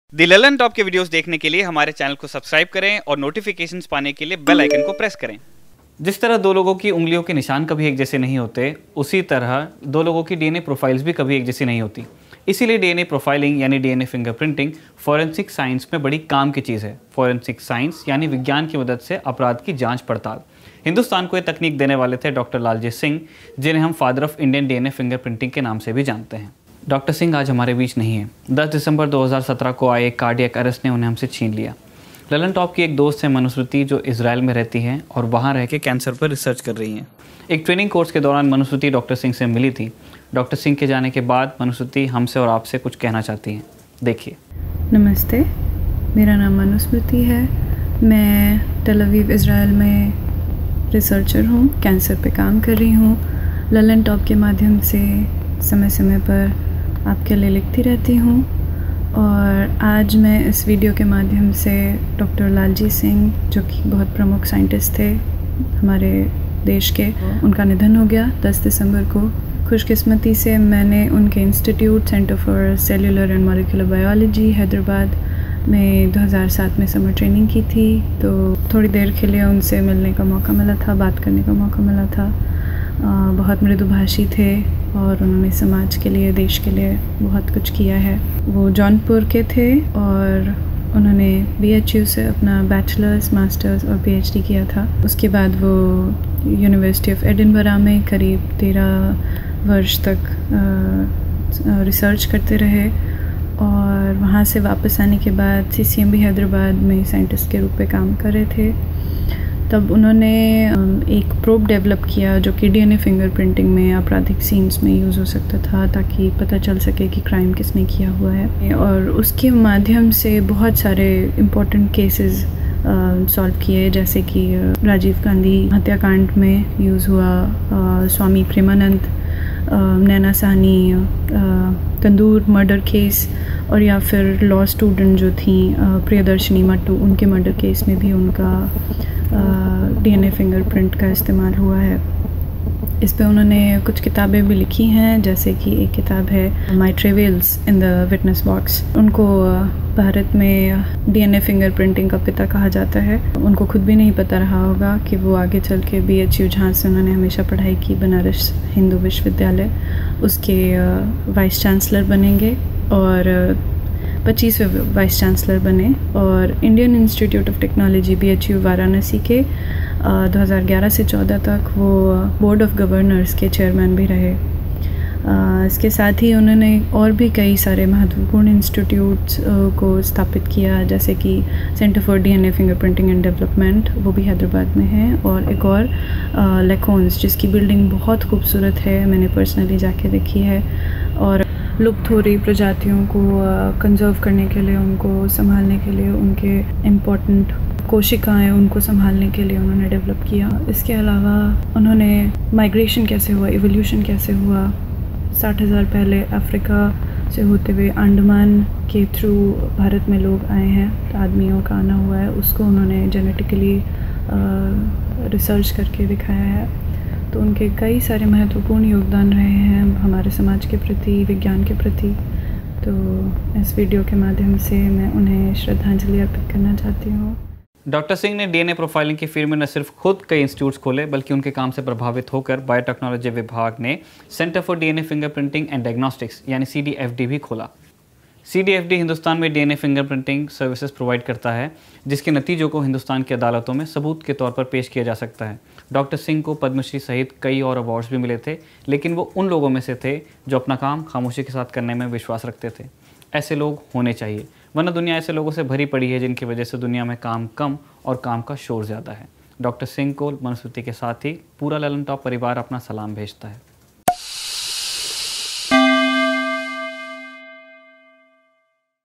भारत में DNA Finger printing के पिता लालजी सिंह की कहानी | The Lallantop
Story of Dr. Lalji Singh, the Father of Indian DNA Finger Printing. Dr Singh pioneered research in DNA profiling in India and opened a number of institutes like Centre for DNA Fingerprinting and Diagnostics and Laboratory for the Conservation of Endangered Species (LaCONES).
Executed By: Varun Sharma